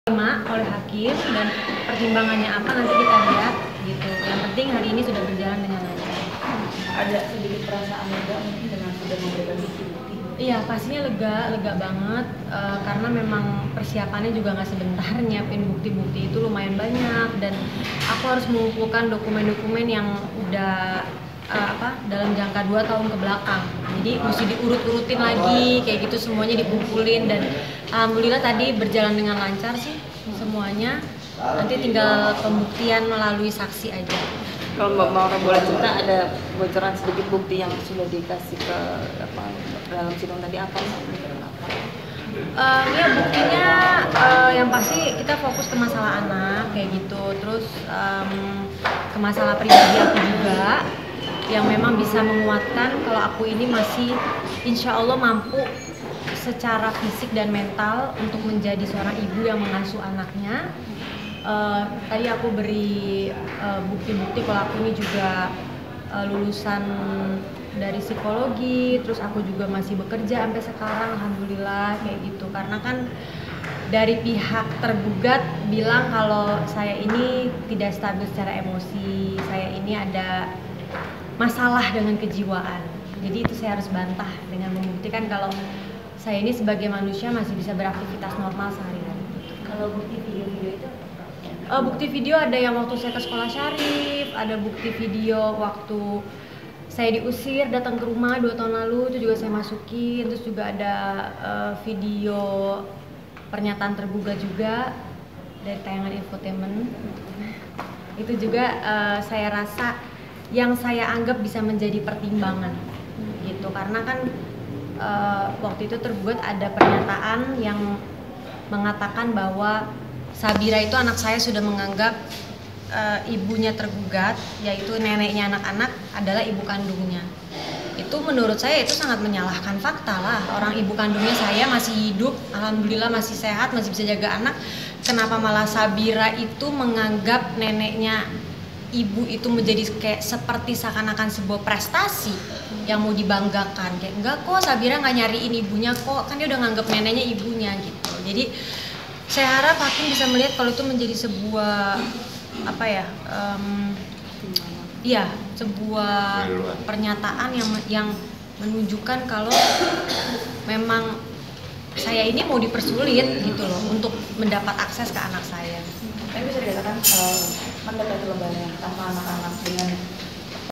Demi Dapat Hak Asuh Anak, Tsania Marwa Bawa Seabrek Bukti ke Pengadilan
Pesinetron Tsania Marwa membawa 25 barang bukti untuk diserahkan dalam sidang lanjutan gugatan hak asuh anak di Pengadilan Agama Cibinong, Rabu (29/5/2019).
Bukti tersebut diharapkan Marwa jadi pertimbangan hakim jika dirinya layak mengasuh ana-anak yang kini dalam kendali mantan suaminya, Atalarik Syah. Selengkapnya dalam video ini